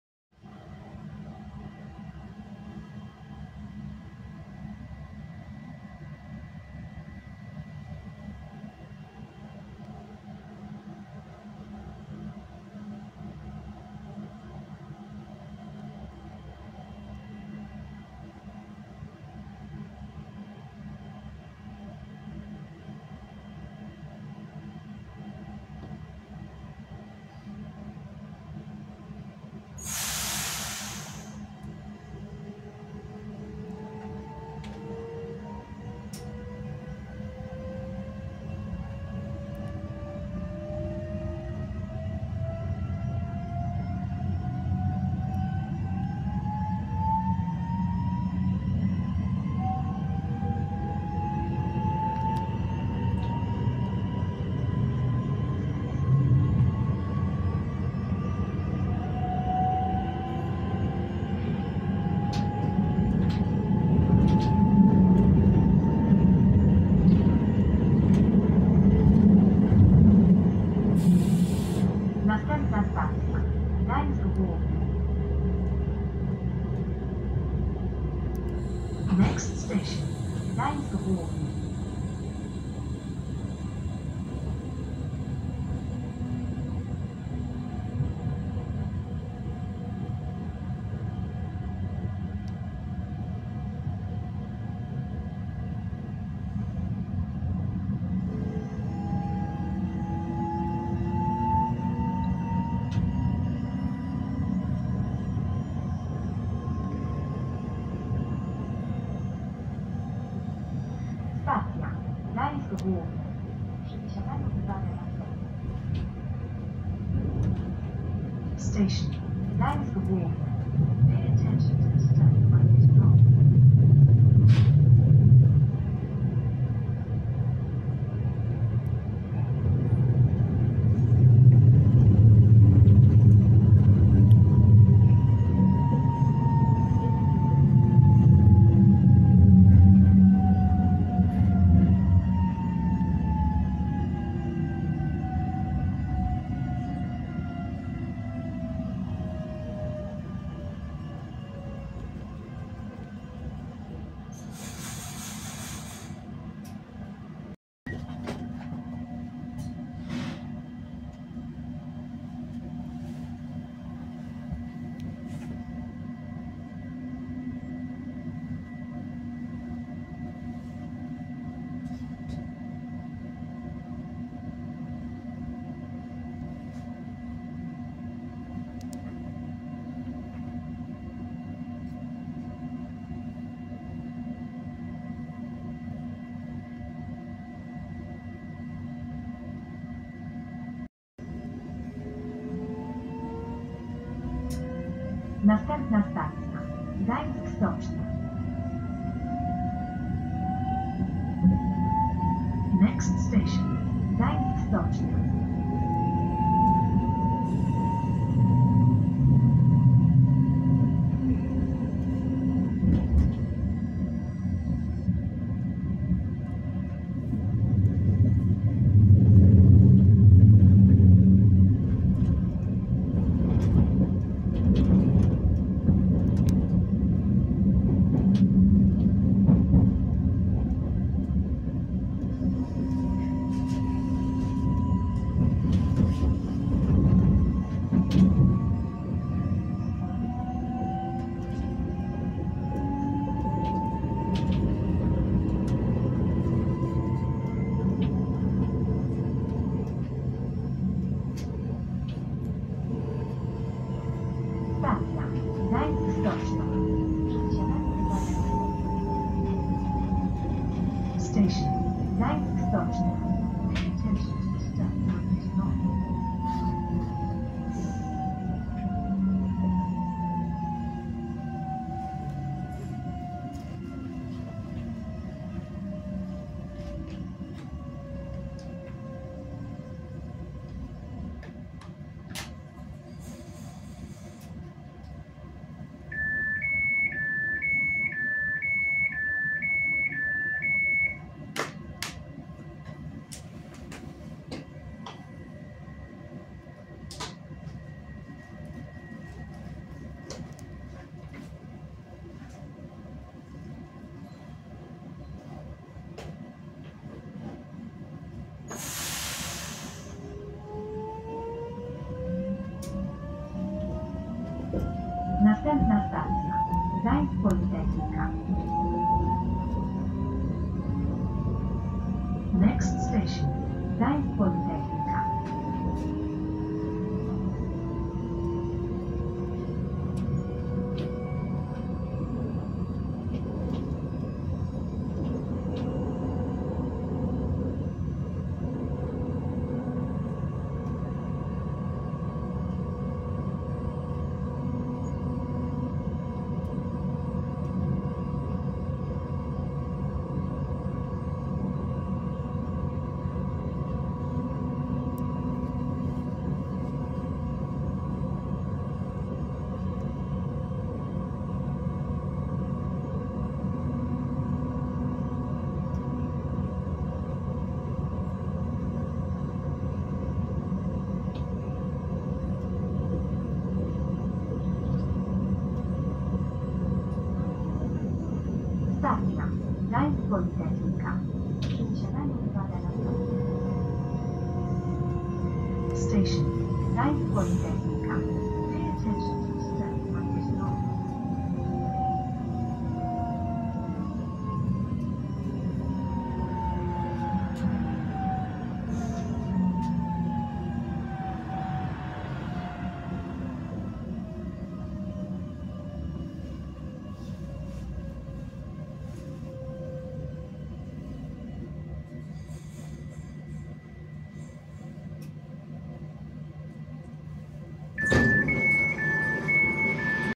[SKM Trójmiasto] Newag Impuls II 31WEbb-016+015 - linia S1, relacja Gdańsk Śródmieście-Gdynia Cisowa
Przejazd nagrano w 31WEbb-016.

-- TRASA --
0. Gdańsk Śródmieście - stacja początkowa
1. Gdańsk Główny
2. Gdańsk Stocznia
3. Gdańsk Politechnika

mkmgdańsk 2024